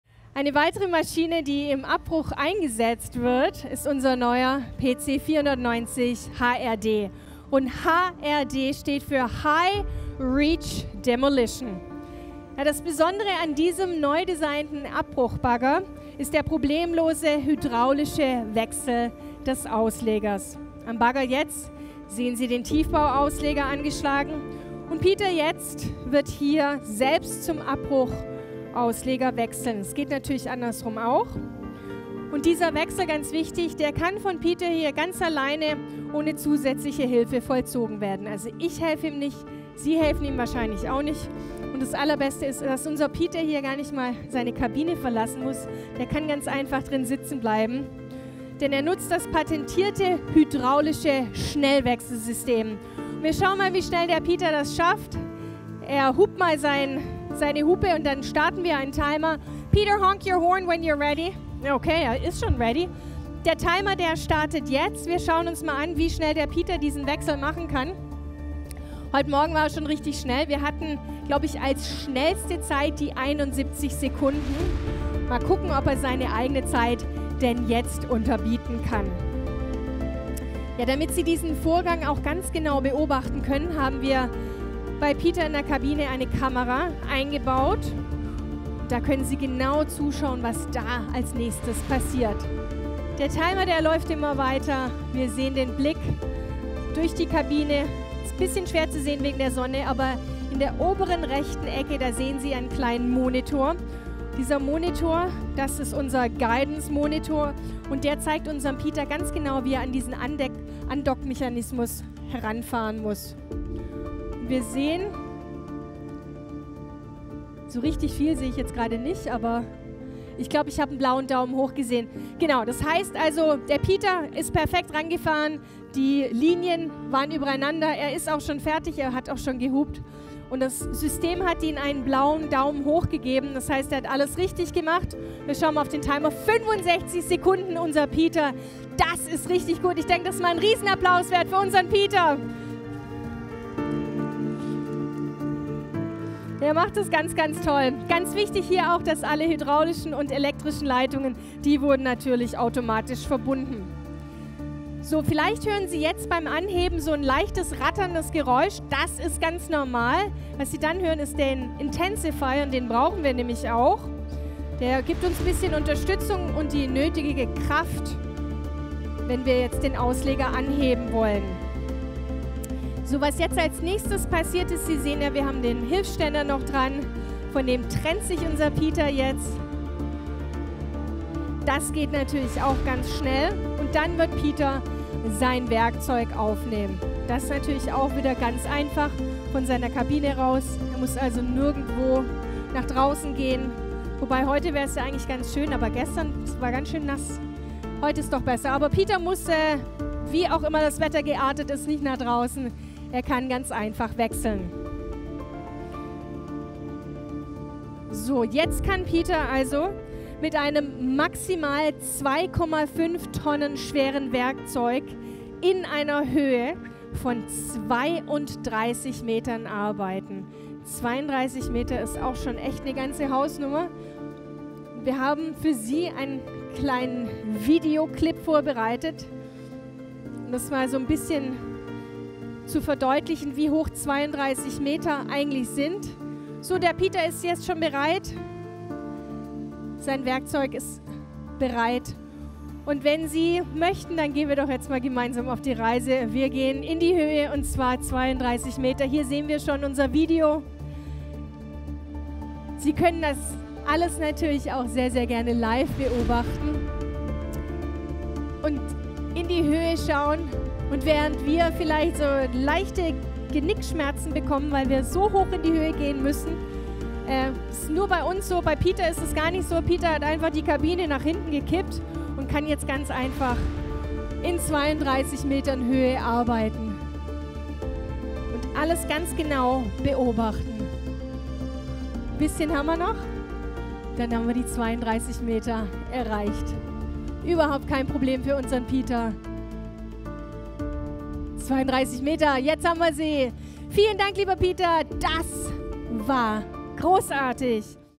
K100-Ausleger-Wechselsystem - Komatsu auf der bauma 2022
Mit dem neuen K100-Ausleger-Wechselsystem können Sie die Auslegerkonfiguration von der Fahrerkabine aus ändern – ohne jegliche Hilfe von außen.
Eine hochentwickelte Mensch-Maschine-Schnittstelle (HMI), die in den Touchscreen der Maschine integriert ist, führt Sie durch den Wechselvorgang der Arbeitsausrüstung. Alle Verbindungen – mechanisch, hydraulisch und elektrisch – werden automatisch hergestellt. Währenddessen bleiben Sie sicher in der Kabine. Es sind keine Mitarbeiter erforderlich, die in größeren Höhen mit schwerer Ausrüstung hantieren.
K100 ist zu 100% von Komatsu entwickelt, hergestellt und unterstützt.